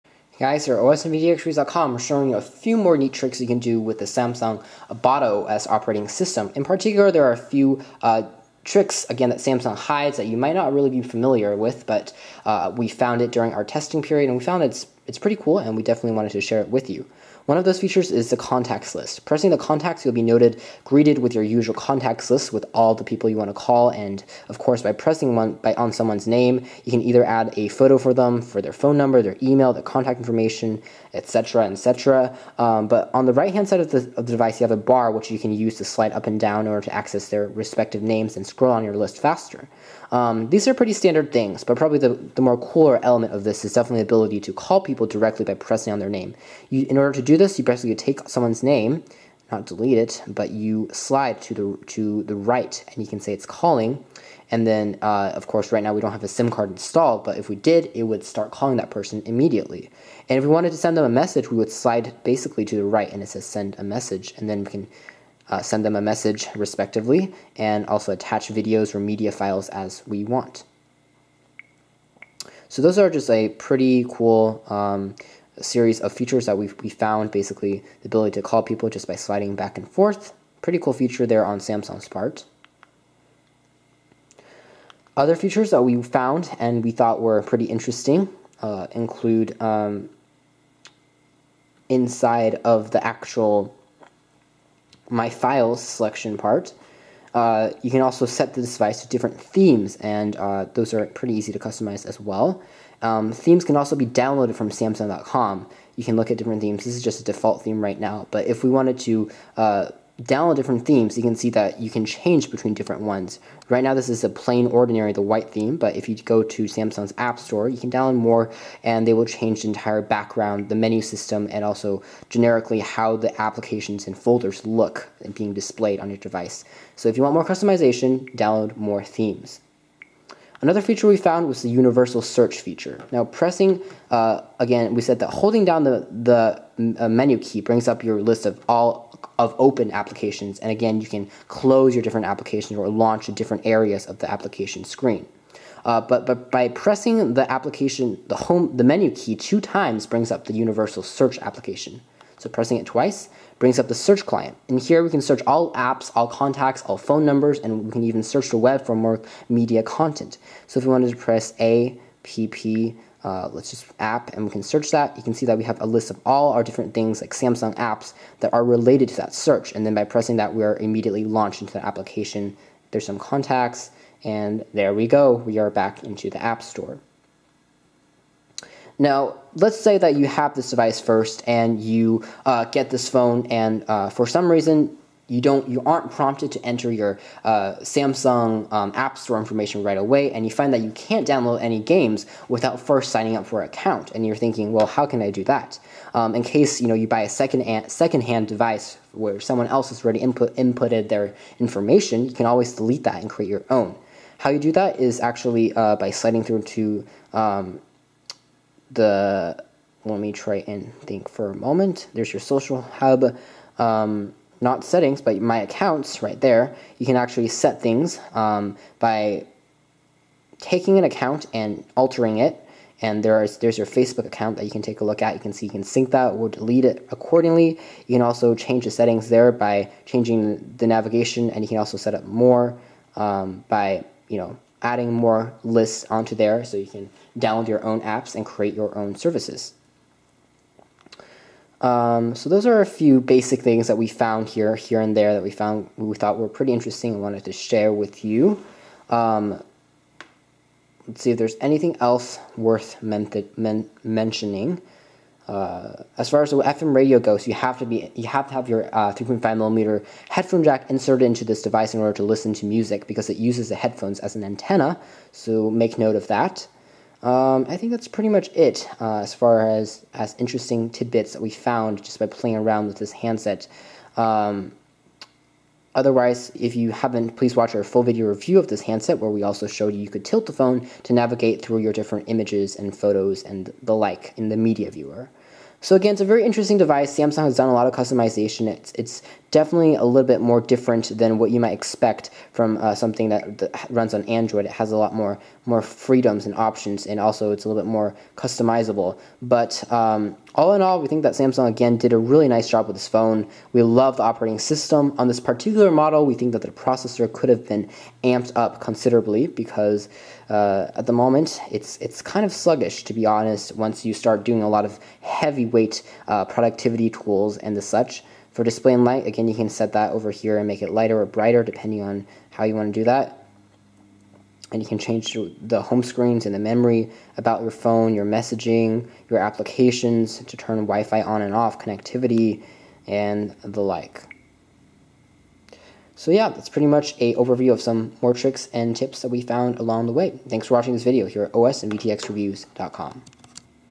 Samsung Bada OS Tricks
Samsung's unique Bada OS features some pretty cool hidden features that many users are unaware of. This video demos some of those neat tricks and tips that can enhance your experience with the Samsung Wave smartphones!